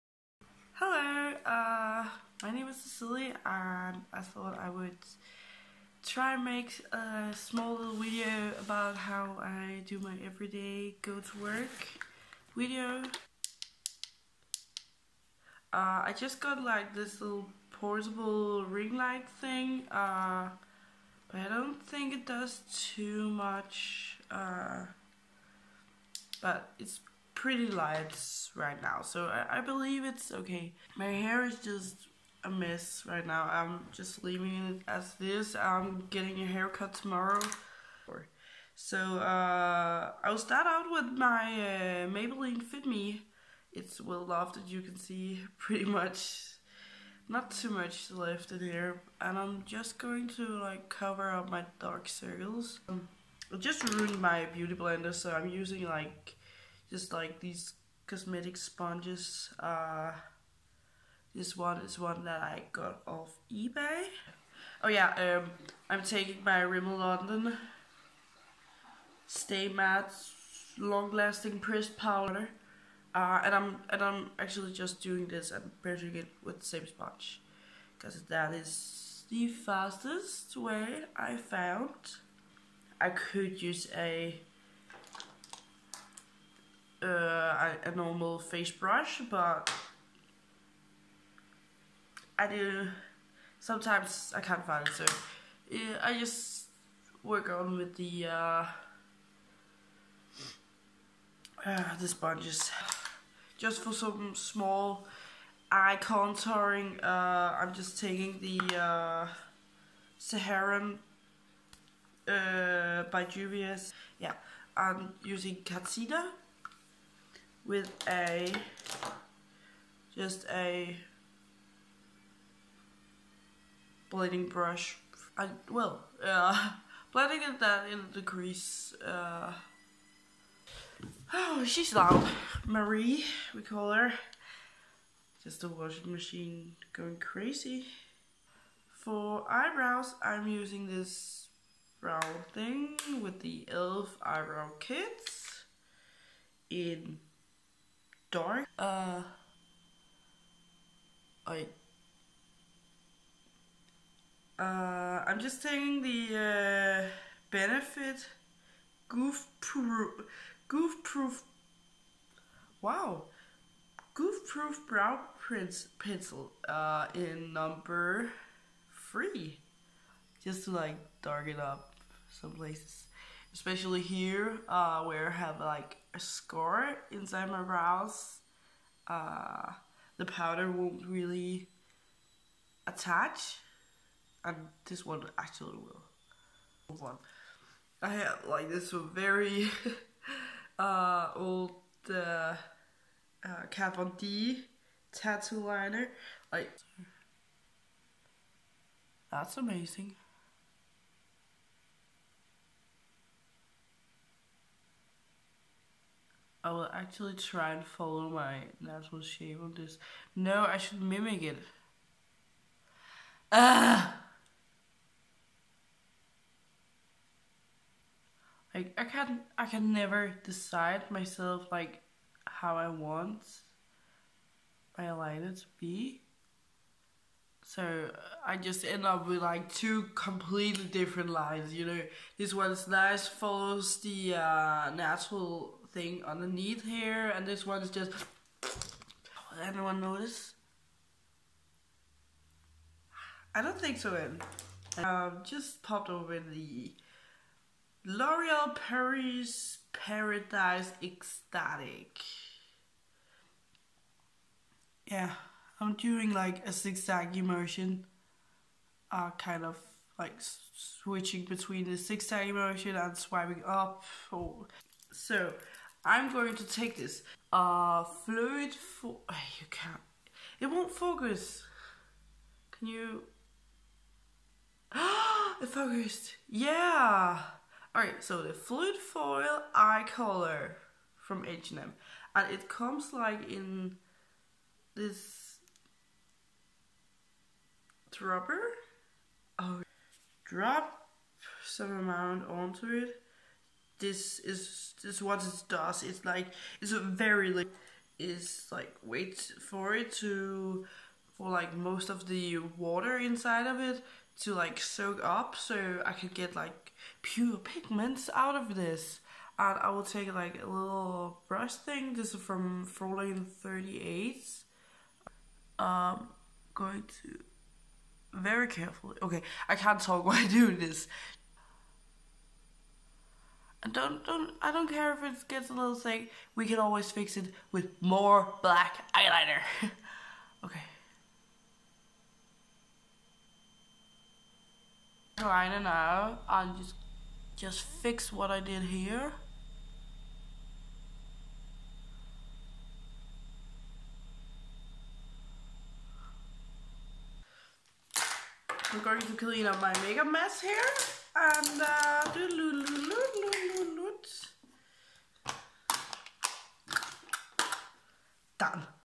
My everyday work makeup with a little extra glam.
Sorry for the washing machine going cray in the background.